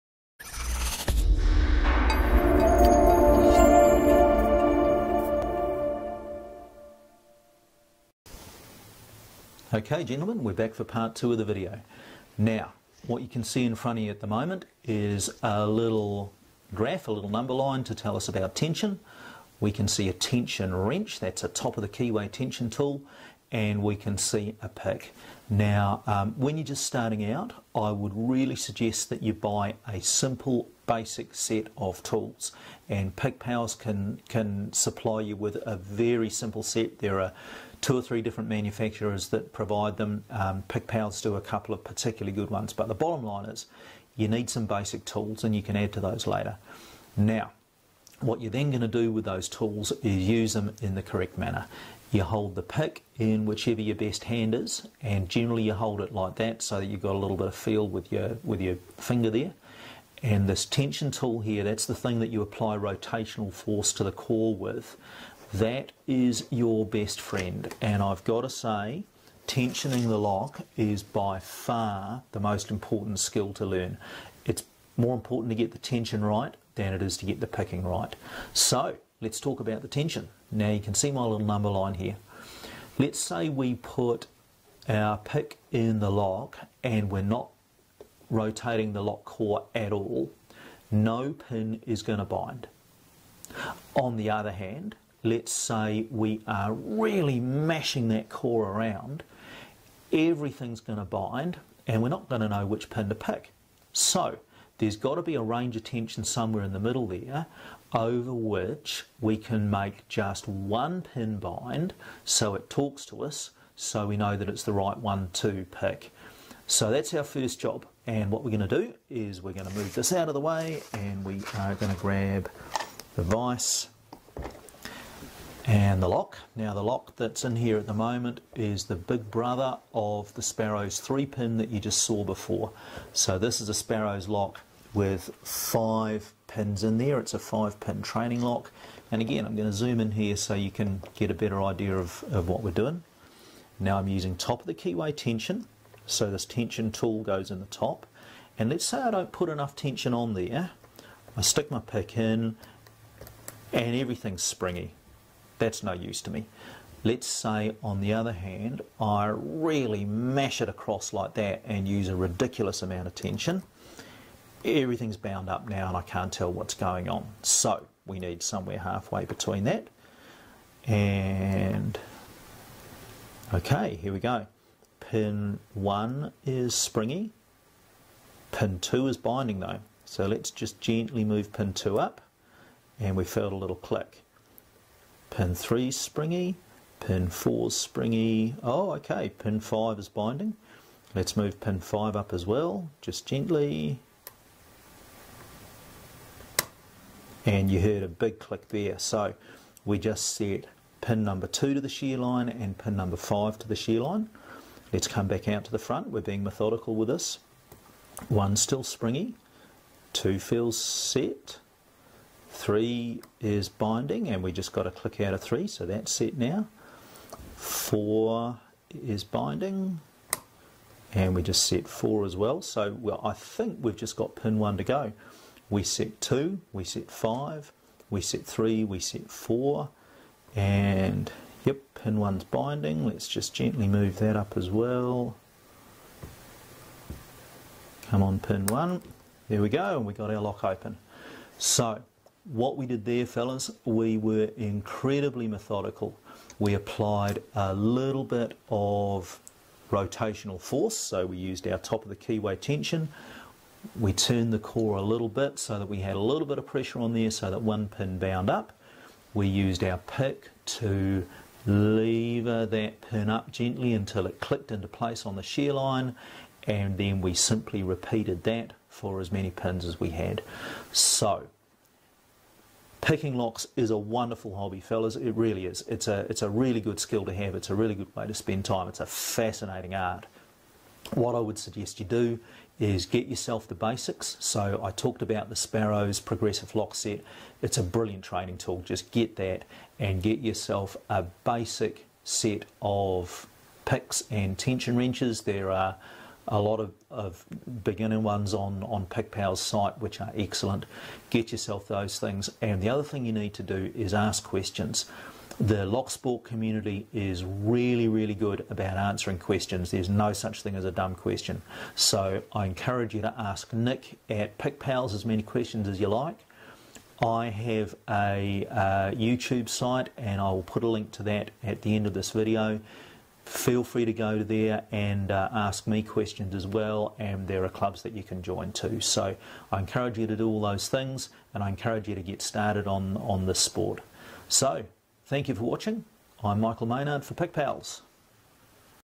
Pickpals Lock Picking Beginners Guide Part 2: How to apply Tension to a Lock
Tension is the MOST important part of lock picking and is often over looked. If you are getting stuck with lock picking, generally it is related to your tensioning. Get this right and PRESTO!

In this video Michael talks about how to tension a lock and how tension applies to lock  picking.